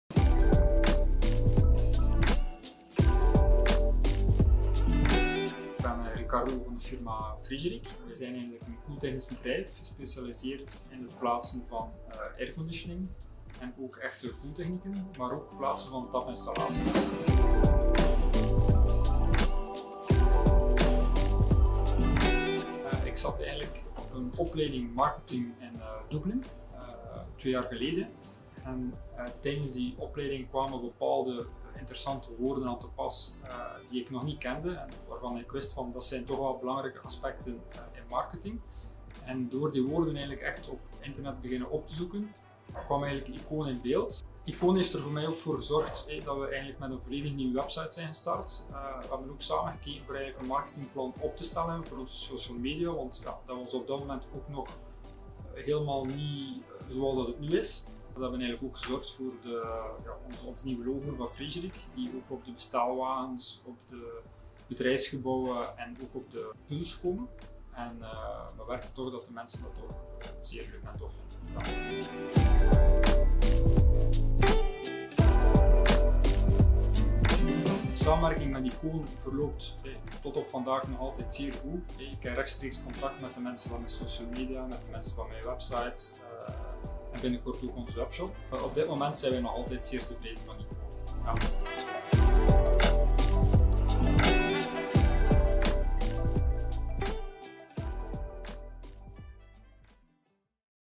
Fregiric koos voor ikoon als full-service communicatiepartner
Als je op zoek bent naar échte kwaliteit voor jouw koelingsystemen, dan klop je best eens op de deur van Fregiric. Ook onze deur stond open voor deze leuke samenwerking! We zorgden voor een rebranding, waarbij het nieuwe logo en de huisstijl werden verwerkt in de nieuwe website, de bestelwagens en de truien. Ook de opvolging van de social mediakanalen komt van onze hand, volledig in samenwerking en onderling overleg met de klant.